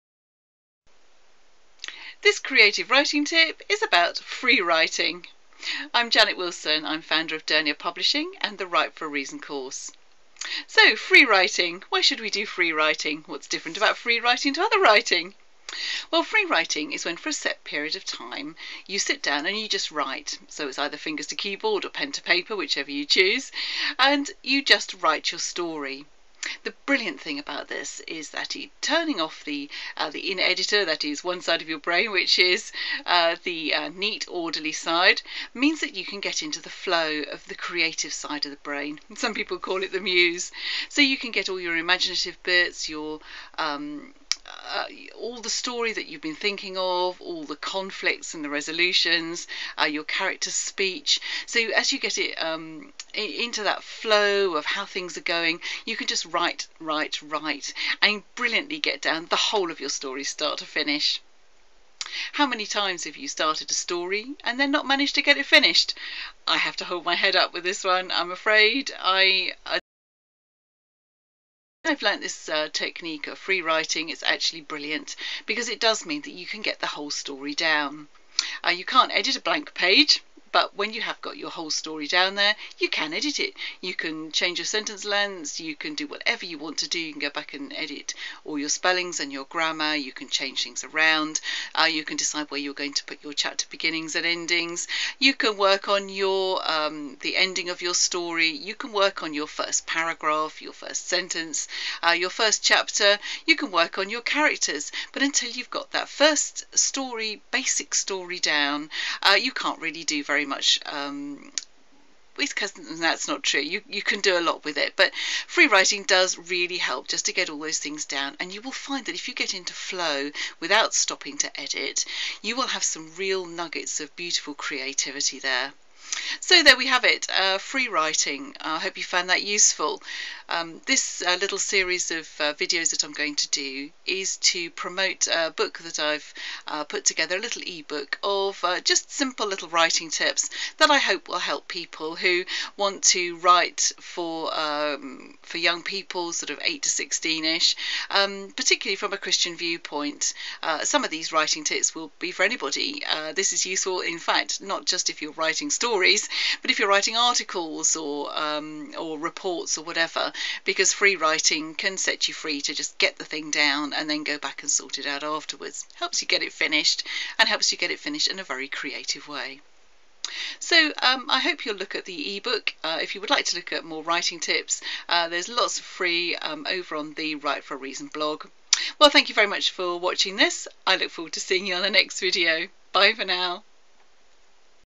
Creative Writing Tip - Freewriting - the how and the why.
Freewriting - what is is and how can you use it in your fiction writing? Janet Wilson, founder of Dernier Publishing and Write for a Reason shows how this technique can be useful in getting your story finished, packed with creativity.